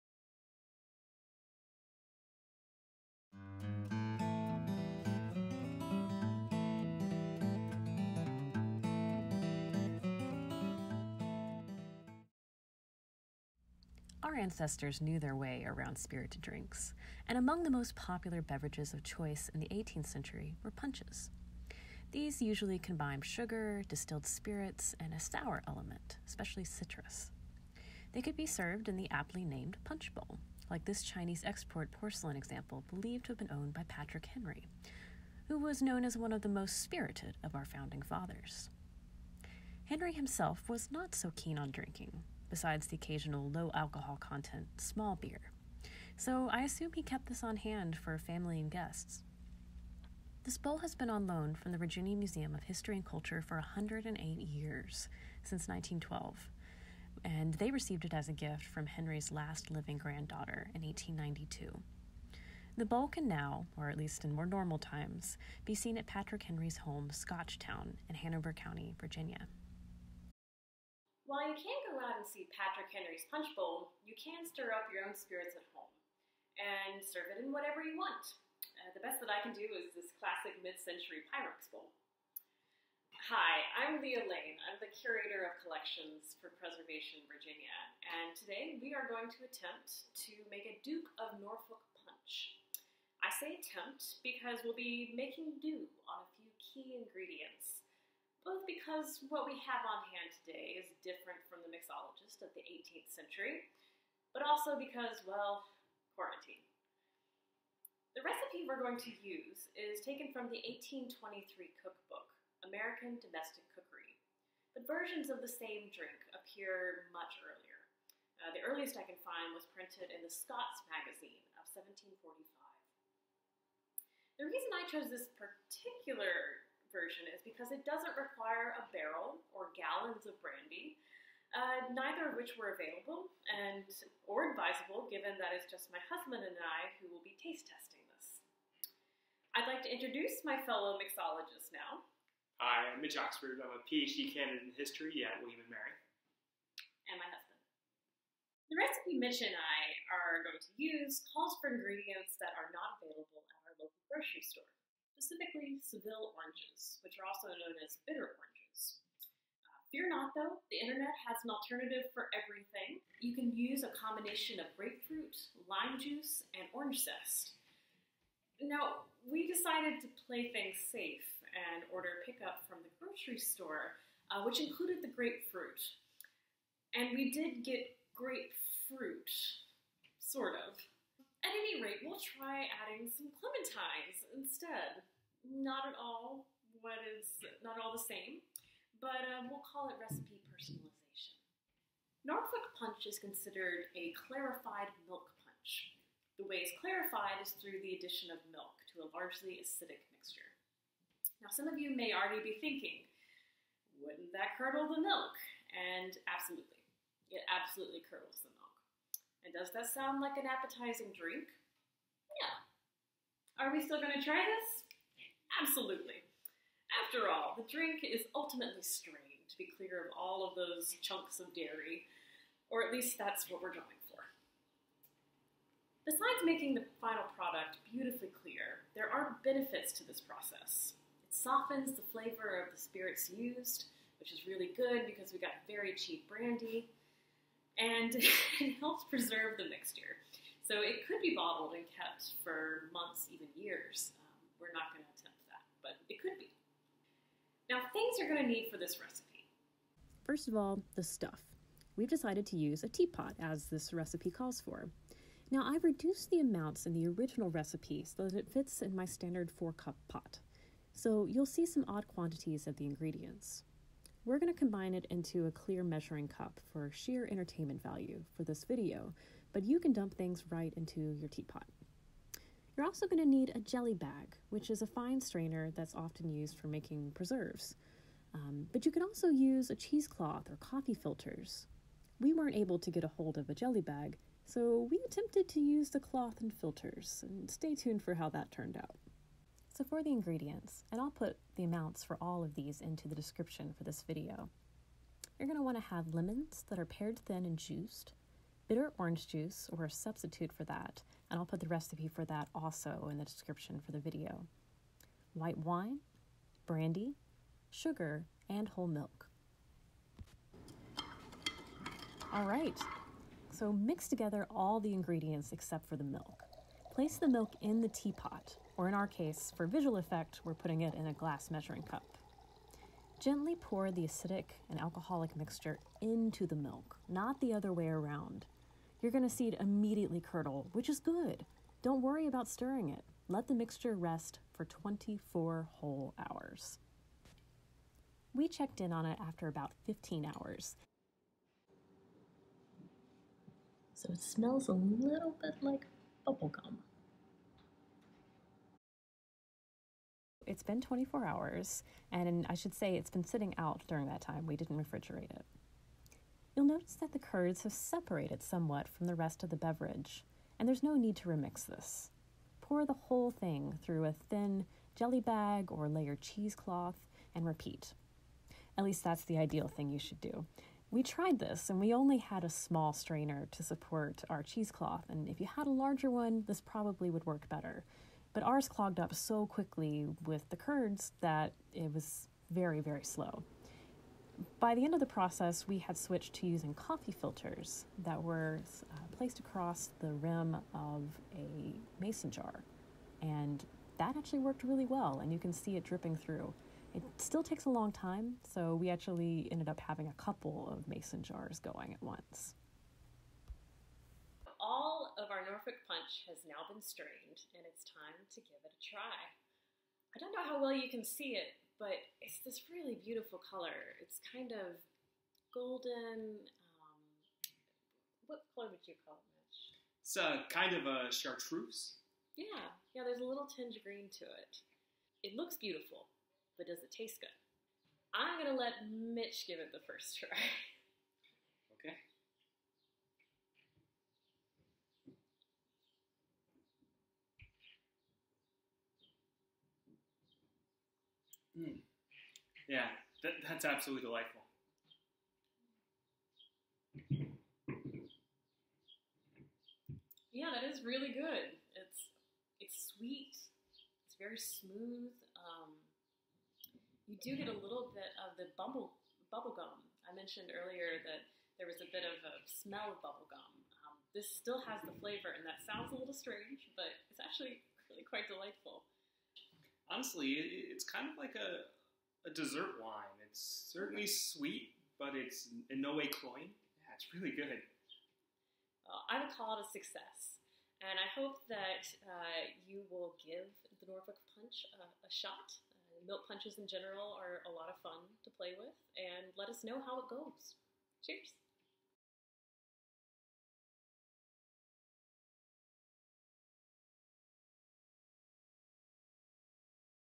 Whipping Up Norfolk Punch with Preservation Virginia
Lea C. Lane, curator of collections, and her husband Mitch show you how to make Duke of Norfolk punch, an 18th century favorite (that's surprisingly good)! Check out the recipe below:

Featured object: Punch bowl, 1760s, China, porcelain, collection of the Virginia Museum of History and Culture, on loan to Scotchtown (Preservation Virginia).

Want to learn more about milk punch? Here are some great resources:

Ben Leggett, editor of the blog “The Drinking Cup,” has a series on the history of punch that are well worth a read:

Dan Sousa of Cooks Illustrated took a deep dive into milk punch, and this article includes some of the best instructions on why and how this process works:

Finally, David Wondrich’s 2010 book Punch: The Delights (and Dangers) of the Flowing Bowl, which is the deepest of deep dives, and won’t disappoint.

Duke of Norfolk Punch

The stuff:
1 teapot – I’ve reduced the amounts in the original recipe so that it fits in my standard 4 cup pot, so you’ll see some odd quantities of the ingredients.
Jelly bag, cheese cloth or coffee filters

The ingredients:
1.5 lemons, pared thin and juiced
¼ cup of bitter orange juice (or substitute – see below)
4/5 or .8 cup of white wine
1 ½ cups brandy (add an extra tablespoon or so)
½ cup granulated sugar (add an extra tablespoon or so)
4/5 or .8 cup of whole milk

Seville/bitter orange substitute:
¼ cup fresh grapefruit juice
1 ¼ tablespoon fresh lime juice
pinch of orange zest

Mix together all of the ingredients in a bowl or measuring cup except for the milk
Pour the milk into the teapot
Gently pour the acidic/alcoholic mixture into the milk – not the other way around
Let the mixture rest at room temperature for 24 hours
Strain mixture using jelly bag, cheese cloth or coffee filter
Repeat until crystal clear! This may take a couple rounds
Put in mason jar or bottle and keep in your cellar or fridge